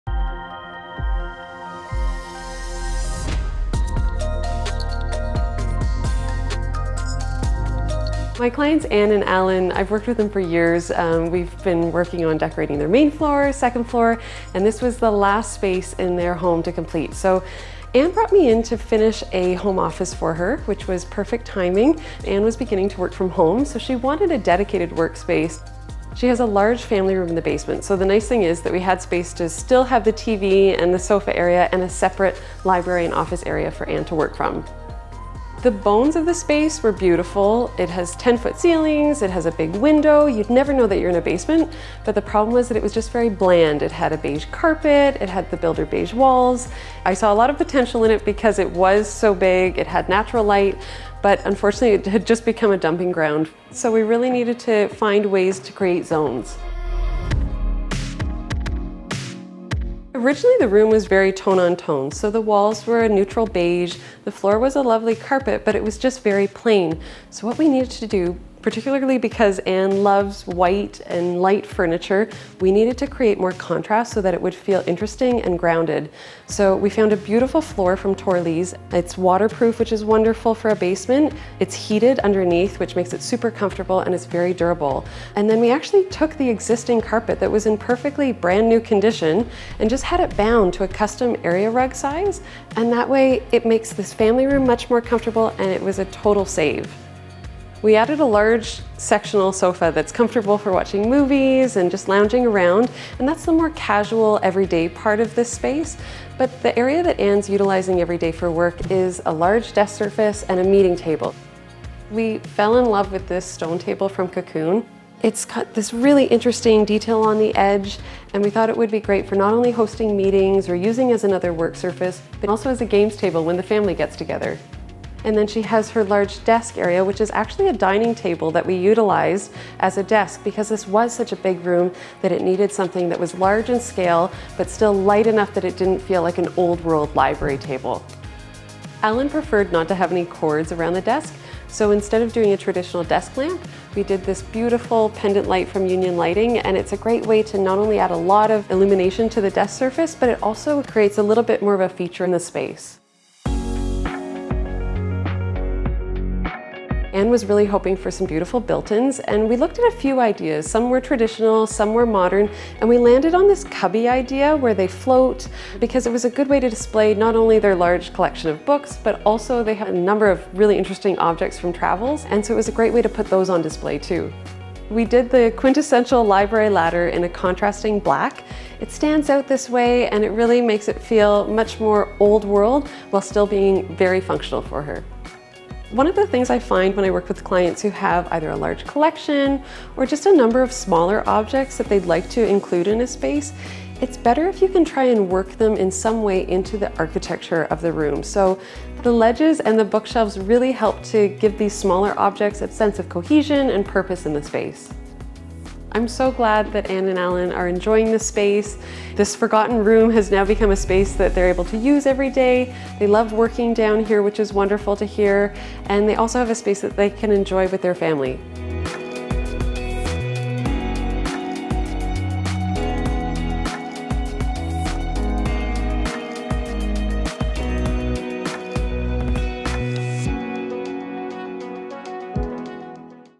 Basement Makeover: The Ultimate Work-From-Home Space
See how designer Jenna Cadieux transformed a bland, beige basement into a hardworking hangout, perfect for work and play. With 10-foot ceilings and lots of natural light, this lower level had plenty of potential — all the space needed was some contrast and savvy organization. Jenna installed Smart Laminate dark wood flooring for visual impact and created two different zones: one area for working from home and another for kicking back and watching a movie with the family. You'll love the new office, complete with a handsome dining table-turned-desk, beautiful built-ins and even a library ladder for a touch of old world flair.

Presented by TORLYS.

SOURCES:
Interior Decorating by Cadieux Design, cadieuxdesign.com
Thumbnail photography by Seth Stevenson, sethstevenson.ca